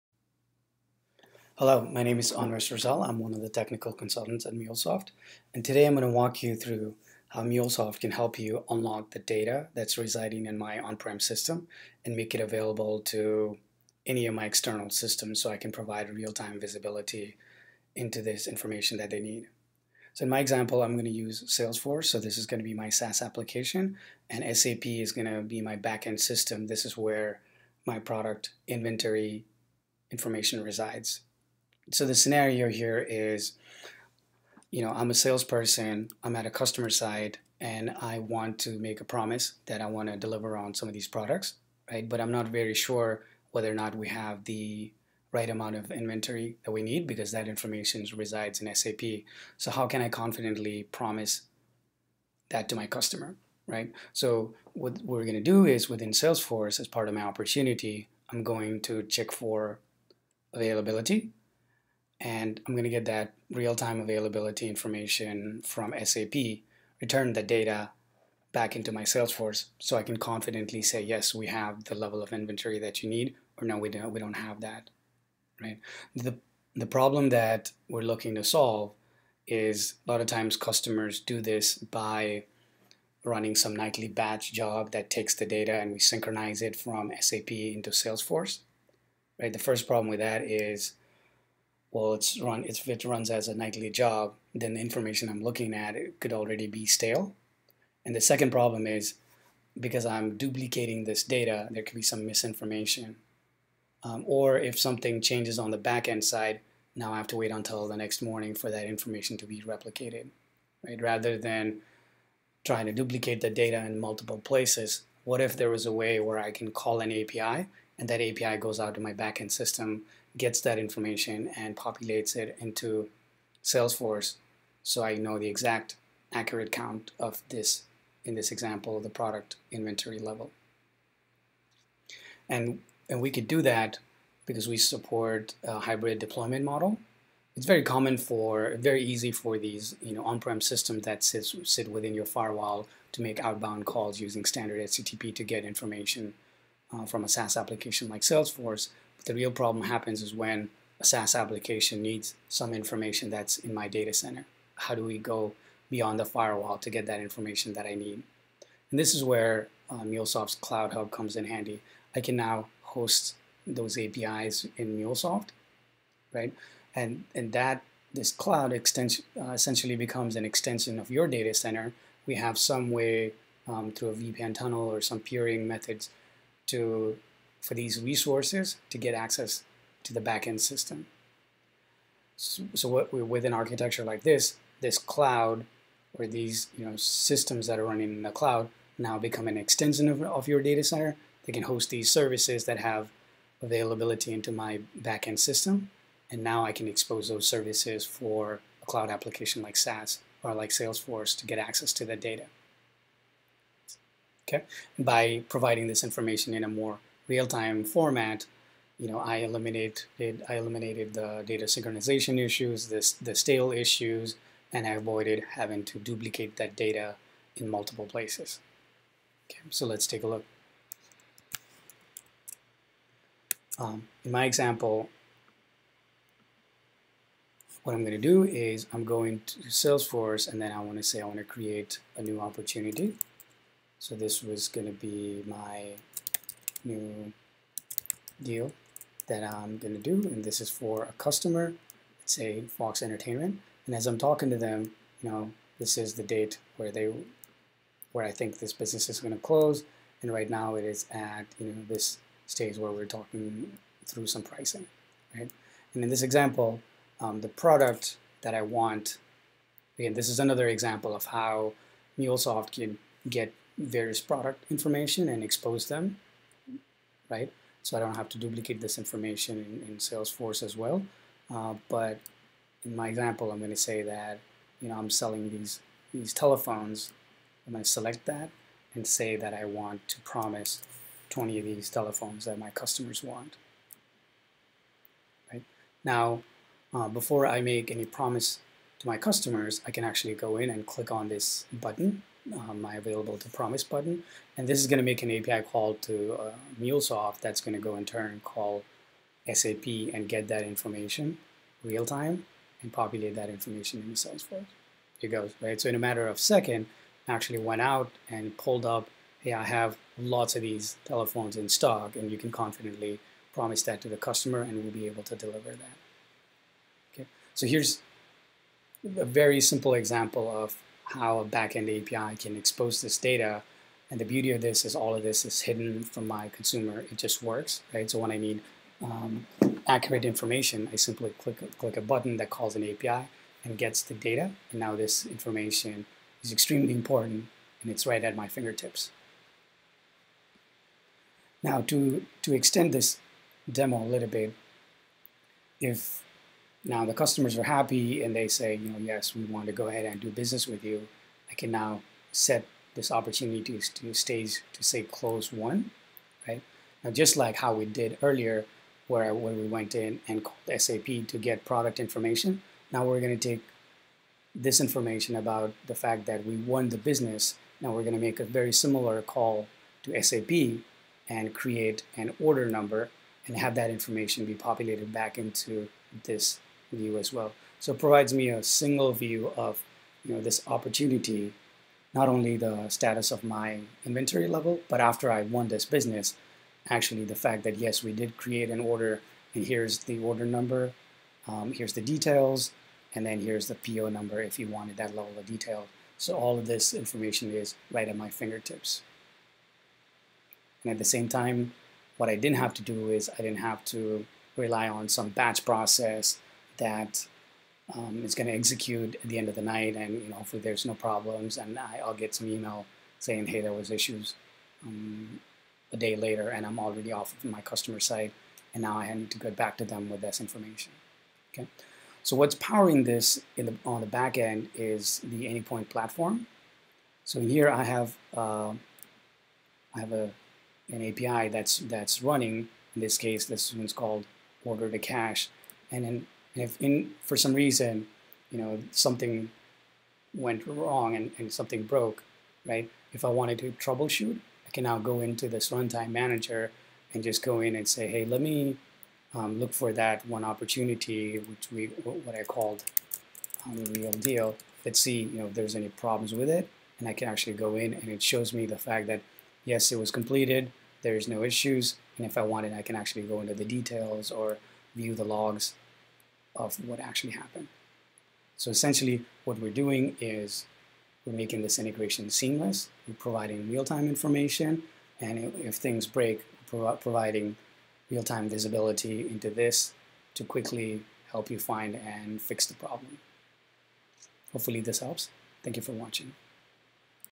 Salesforce to SAP - real time connectivity using MuleSoft
Demo of a micro order-to-cash scenario highlighting the real time connectivity between a Cloud application (Salesforce) to an on-prem system (SAP)